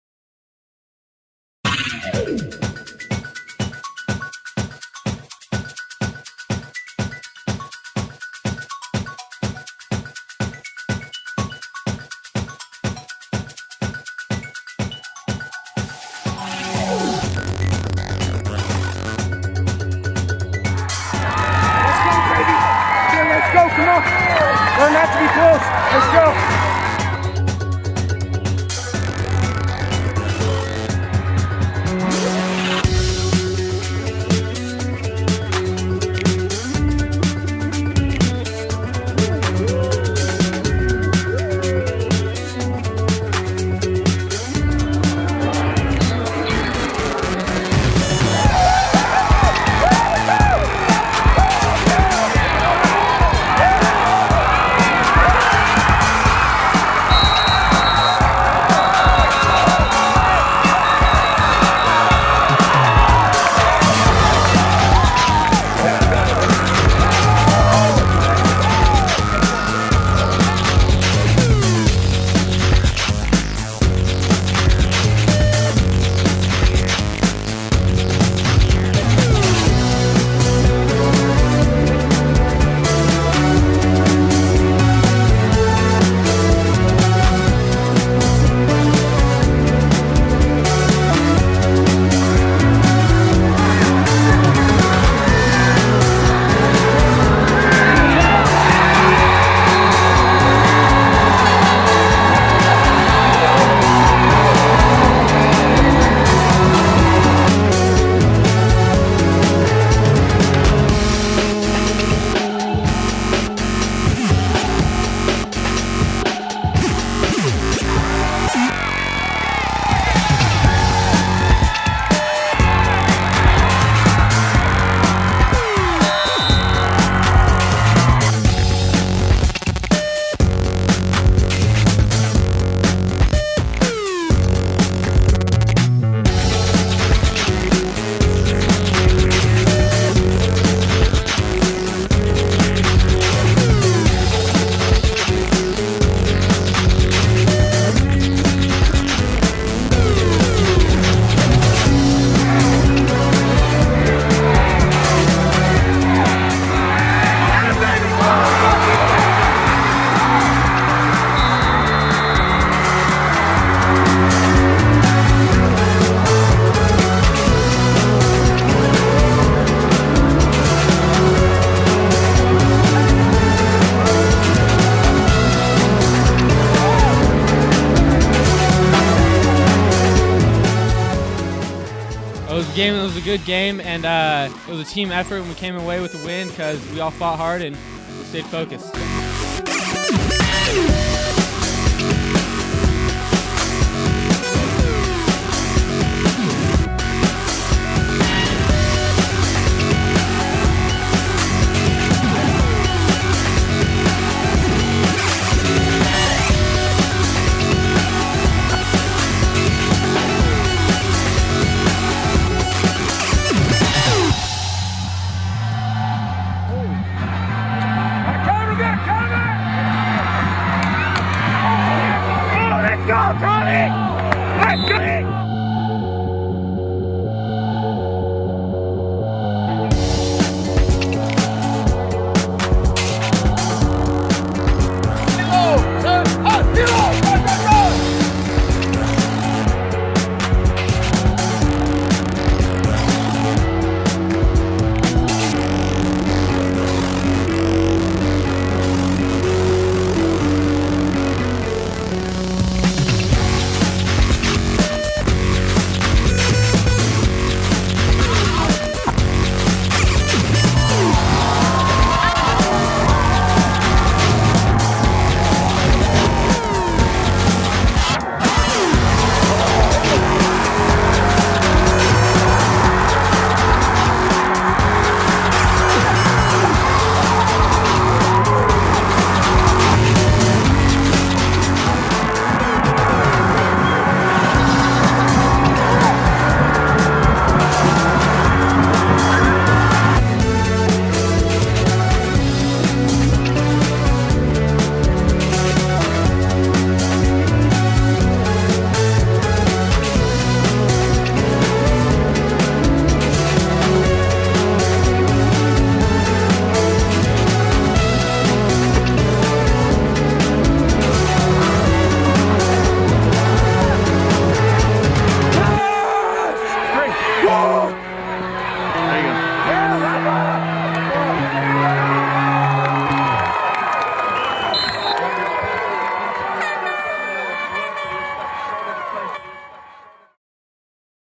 2009 Centennial High Football Team Video Part 4
Golden Hawk Football Individual Athlete Introductions and Highlights. Video footage closeups of each athlete taken from sidelines.